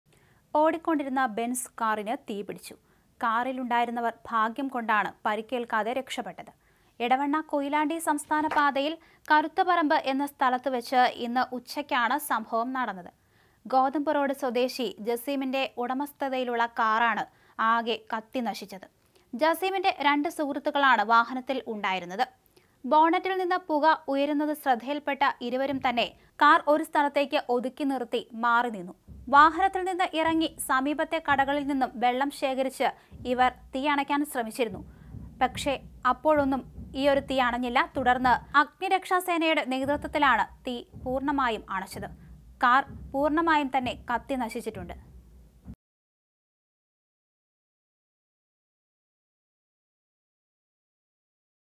കാര്‍ കത്തിനശിച്ചു; യാത്രക്കാര്‍ രക്ഷപ്പെട്ടു | Kozhikode | Benz car Edavanna Koilandi state highway
ബെന്‍സ് കാറിന് തീപ്പിടിച്ചു. കാറിലുണ്ടായിരുന്ന യാത്രക്കാര്‍ പരിക്കേല്‍ക്കാതെ രക്ഷപ്പെട്ടു.ജസീമിന്റെ രണ്ട് സുഹൃത്തുക്കളാണ് വാഹനത്തില്‍ ഉണ്ടായിരുന്നത്.Benzcar Kozhikode # Edavanna